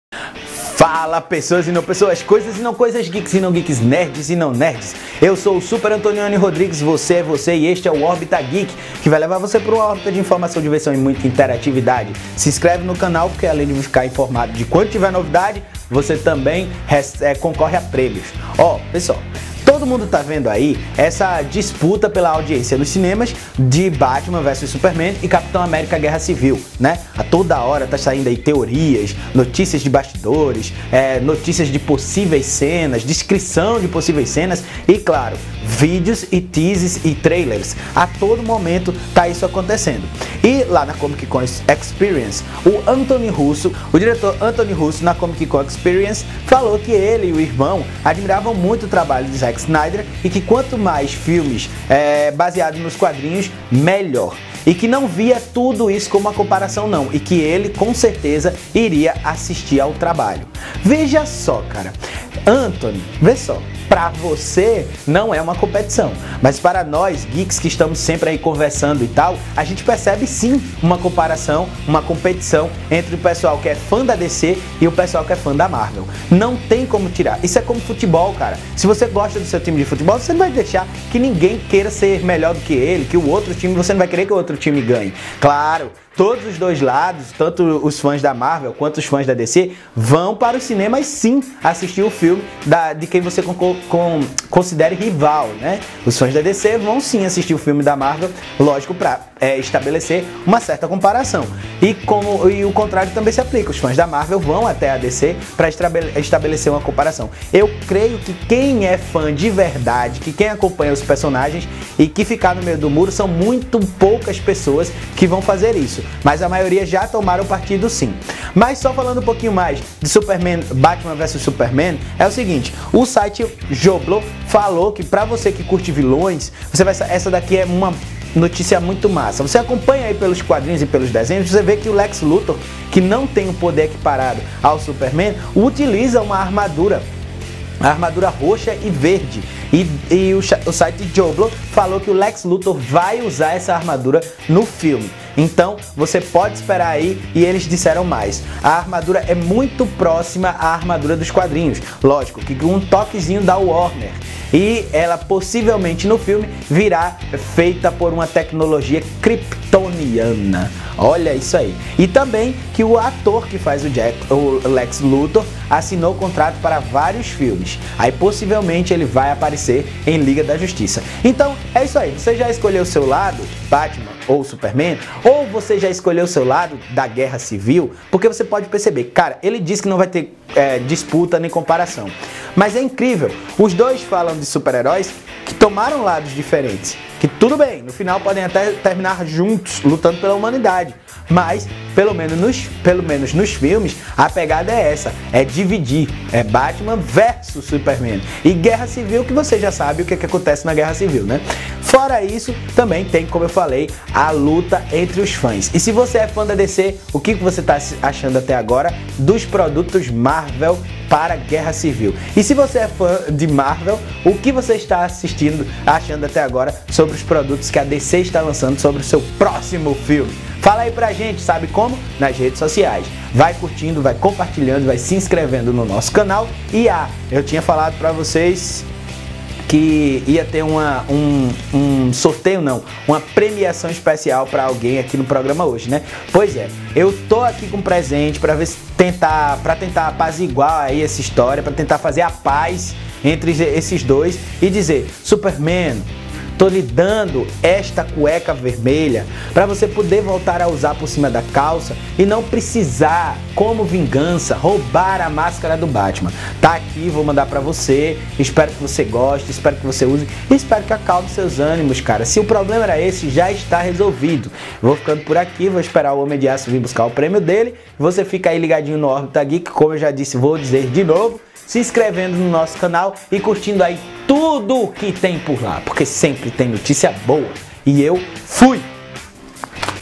ANTHONY RUSSO DIRETOR DE CAPTÃO AMÉRICA GUERRA CIVIL FALA DE DISPUTA COM BATMAN VS SUPERMAN
EITA ! EU DISCORDO UM POUCO DELE ... MAS E VOCÊ ?